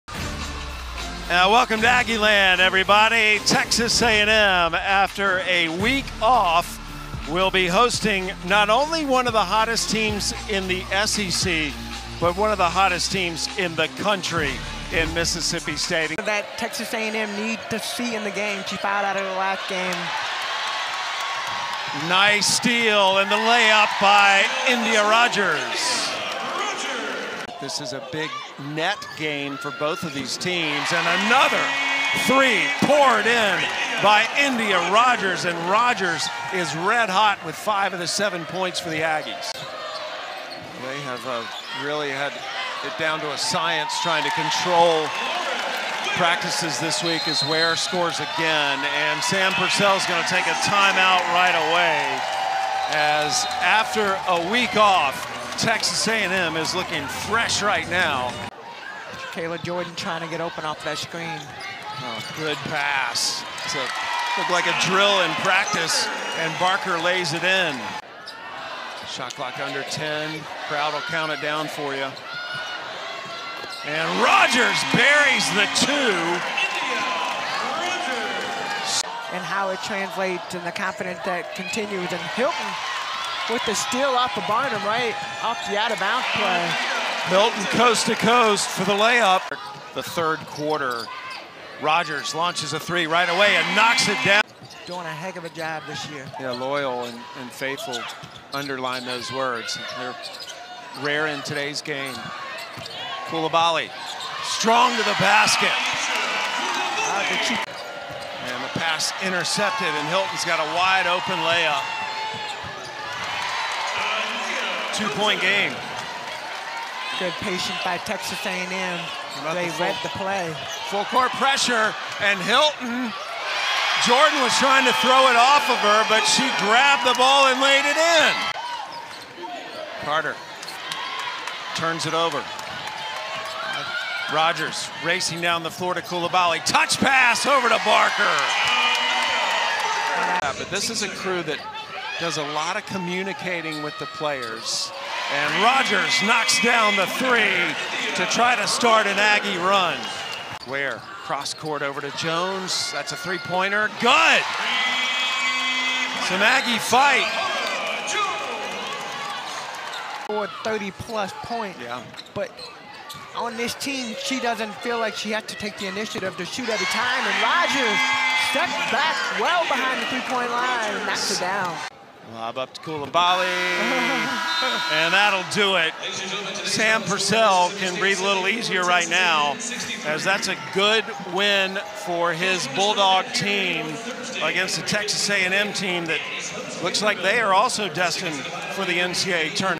Highlights: Texas A&M vs Mississippi State
Highlights of Texas A&M women's basketball's loss to Mississippi State at Reed Arena on February 4, 2024.